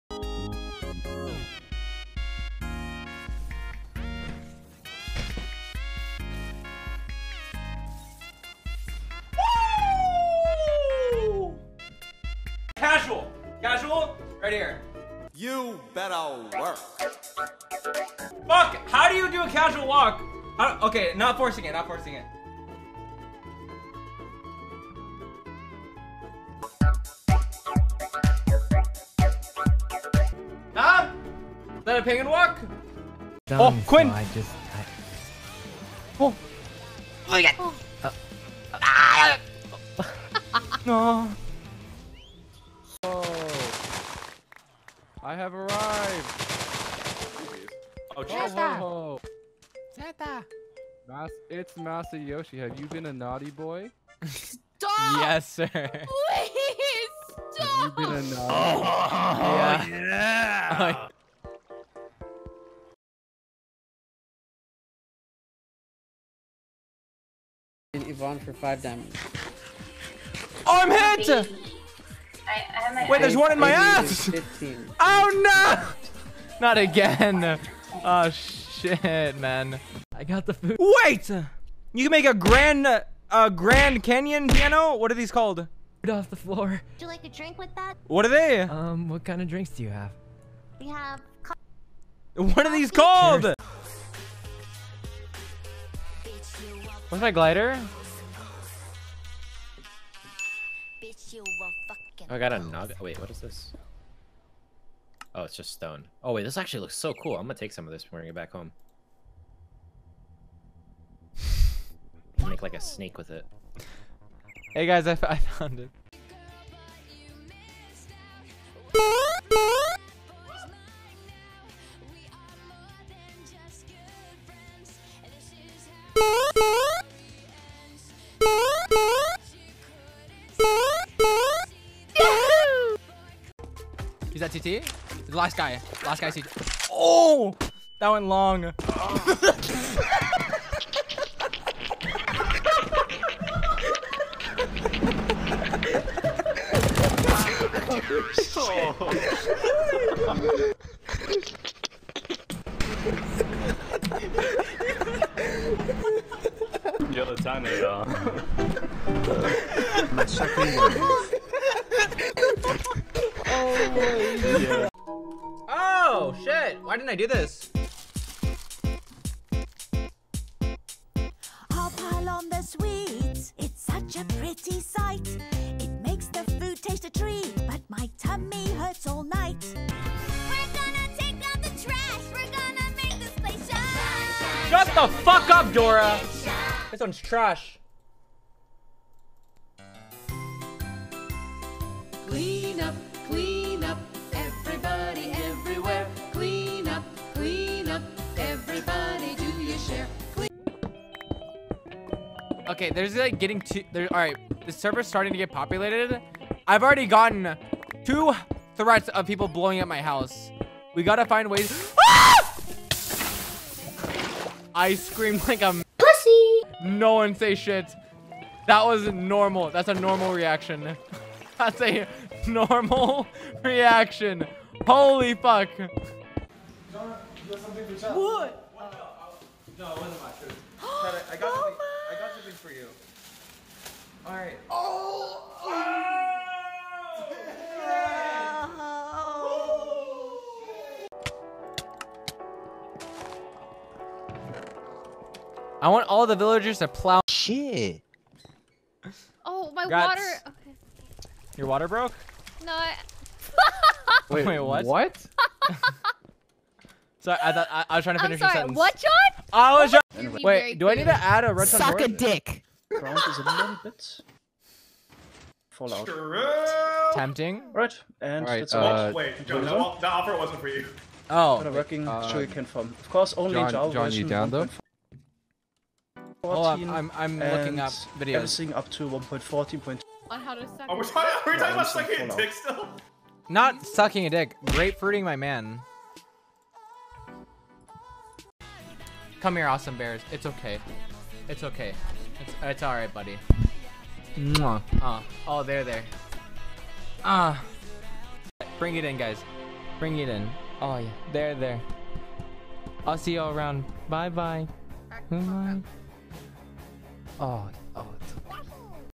NO ONE IS SMARTER THAN HIM ( Masayoshi compliation video)
This video is in honor of awesumbear... he first reached out to me after the first peter park vid and encouraged me to major in film making. Now I will be transferring and switching my major. If you ever watch this video, thank man. We all miss you in the community.

The clips used in this video belong to Masayoshi on twitch. ILY JonJon :)